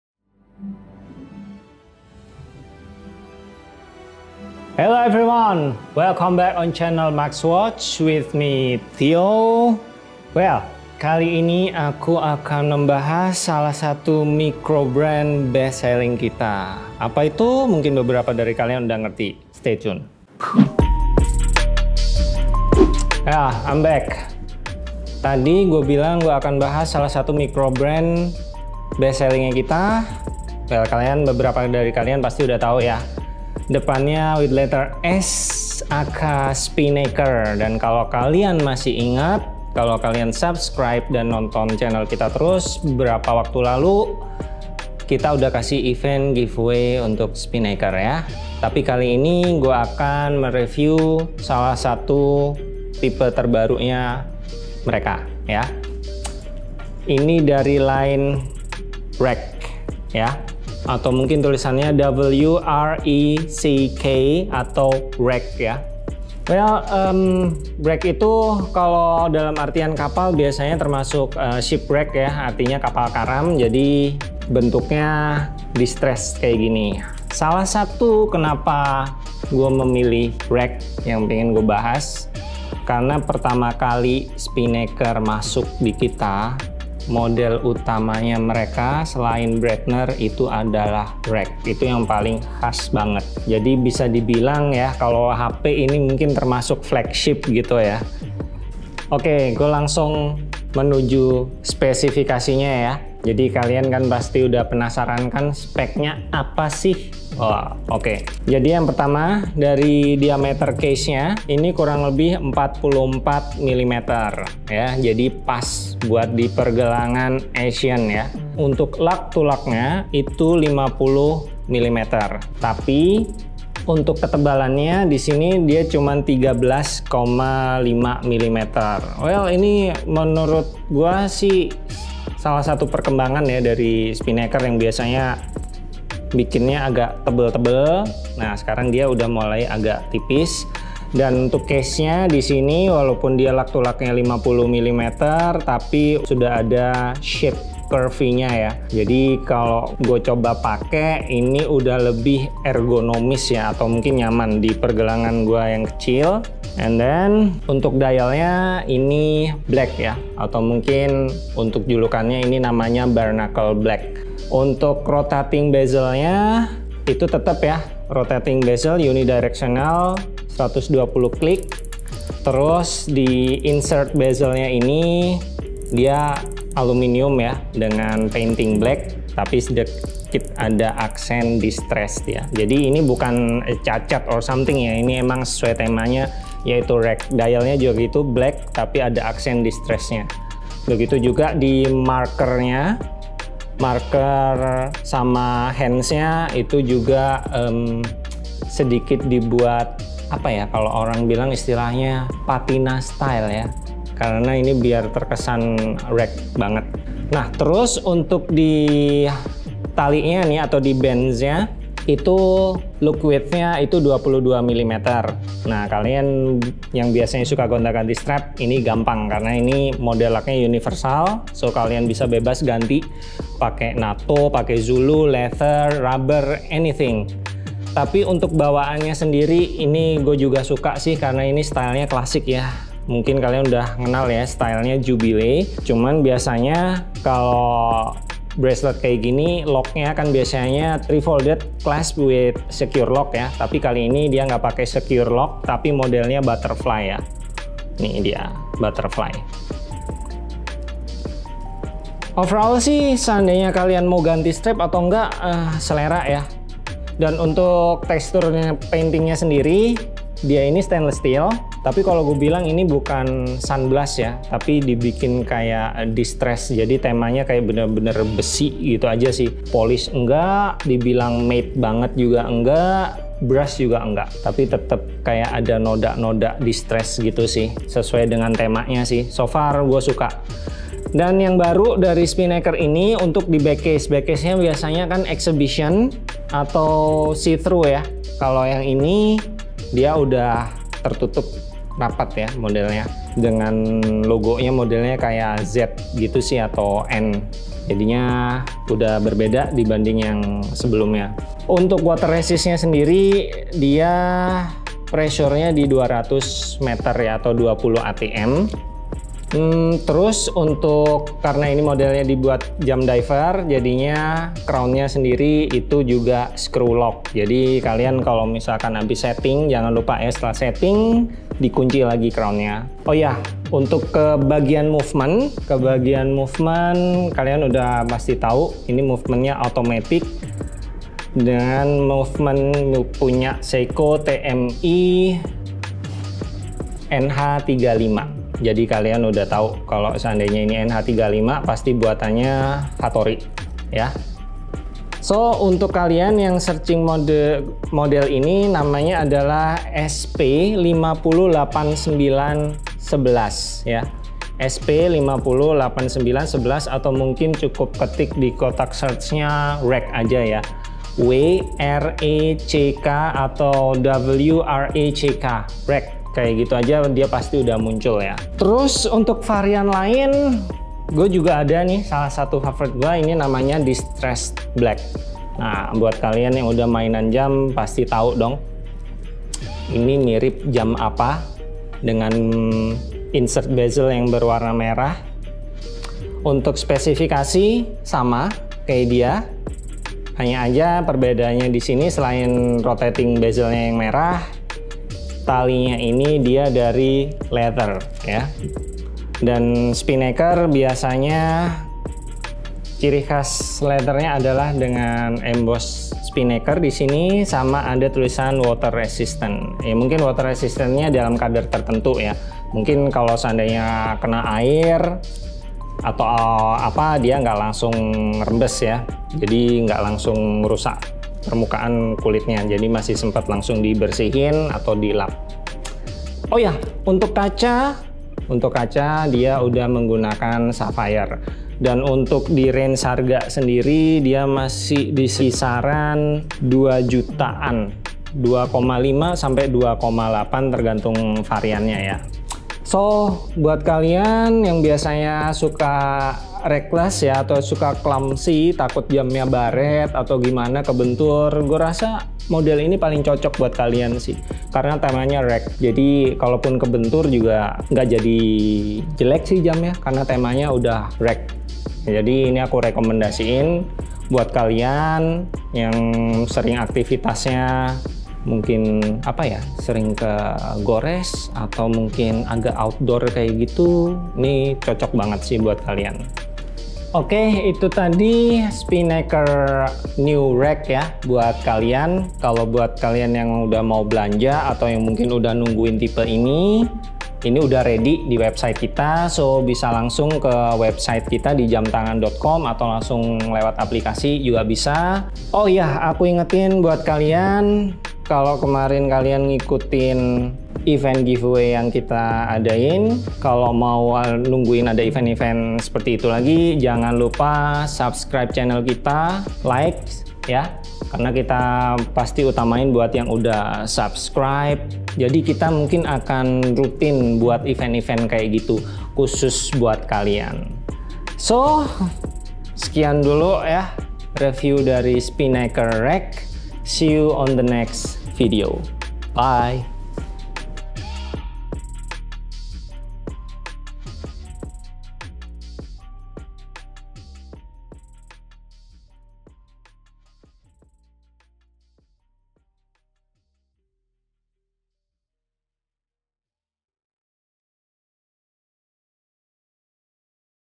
DIBIKIN KAYAK KAPAL KARAM❓❓ KUPAS TUNTAS SPINNAKER WRECK
Tyo kembali lagi ngebahas jam tangan micro brand yang termasuk best seller di Jamtangan.com. Mau tau? Yang direview kali ini adalah Spinnaker Wreck yang unik banget. Apa sih yang bikin jam ini unik? Ternyata desainnya emang sengaja dibuat mirip kapal karam dengan tekstur dan efek besi berkarat. Selain tampilannya itu, kualitas materialnya juga ga main-main bro. Kira-kira apa aja fitur dan kelebihannya? Yuk, cari tahu di videonya ya!